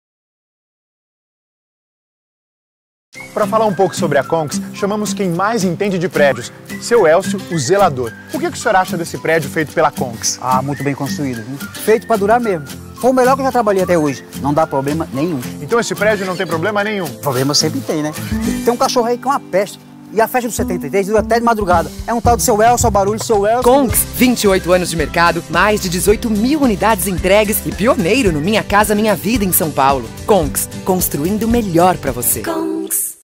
Especialistas falam.
Sabe quem é a pessoa mais indicada para falar sobre um prédio? O Zelador, lógico! É ele quem cuida do empreendimento e ouve todas as reclamações. Mas não quando é um Conx. Aí é só elogio. Duvida? Veja o vídeo.